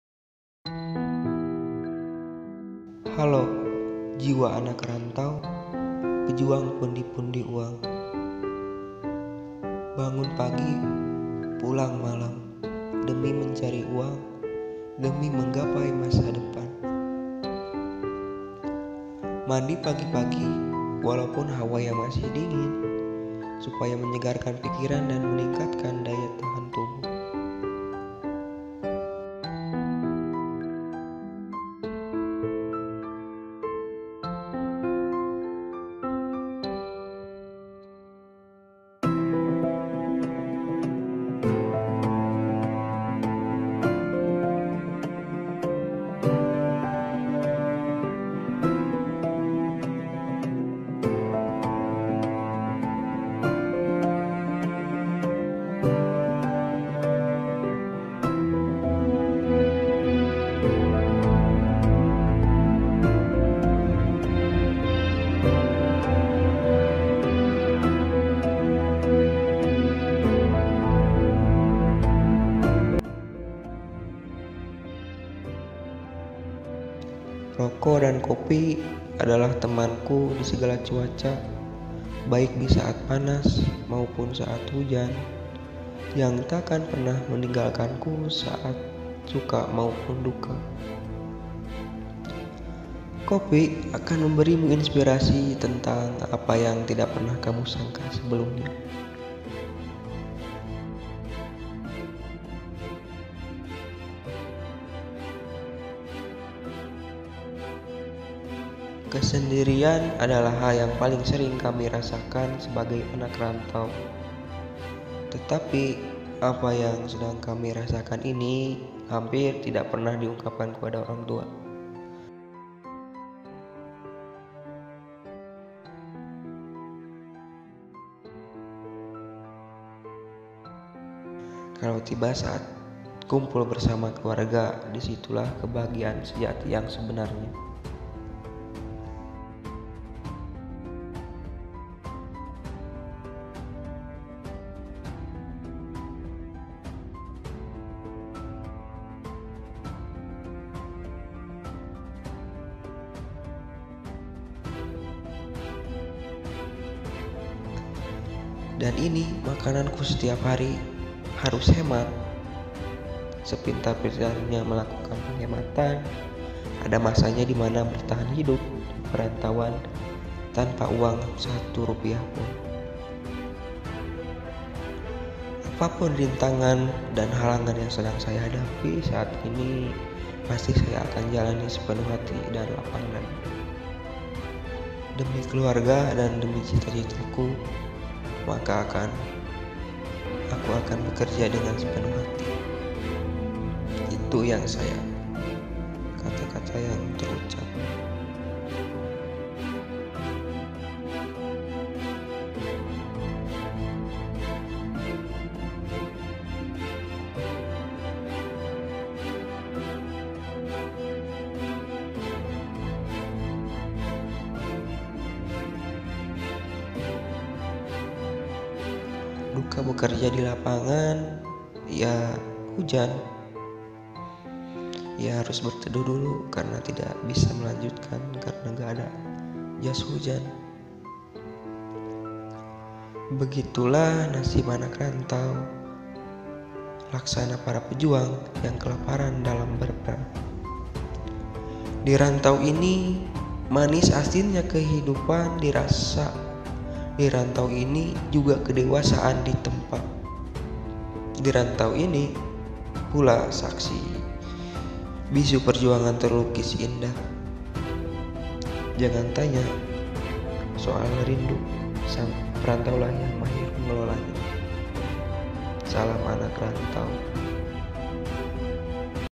Anak Rantau - Suka Duka di PERANTAUAN
Kesendirian adalah hal yang paling sering kami rasakan sebagai anak rantau. Tetapi apa yang sedang kami rasakan ini hampir tidak pernah diungkapkan kepada orang tua. Kalau tiba saat kumpul bersama keluarga, di situlah kebahagiaan sejati yang sebenarnya